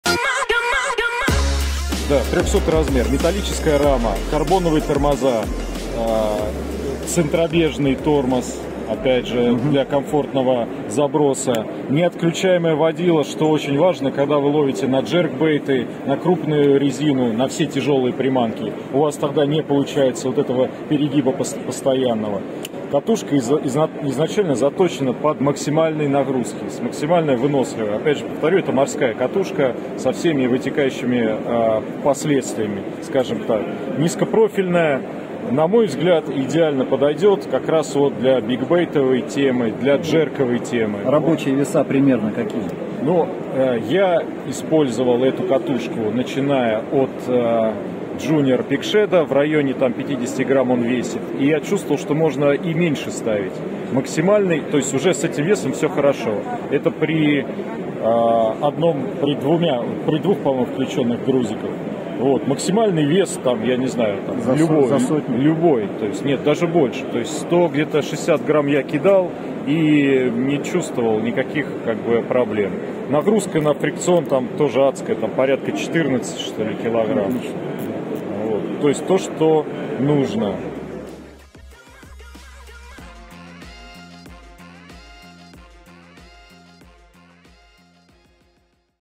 Обзор мультипликаторной катушки Tica Titanclaw TC301 Baitcasting Reel
Компания TICA долгое время (с 1965г.) изготавливала запчасти и узлы для Daiwa С 1993 г стала самостоятельной маркой с массой нароботок и технологий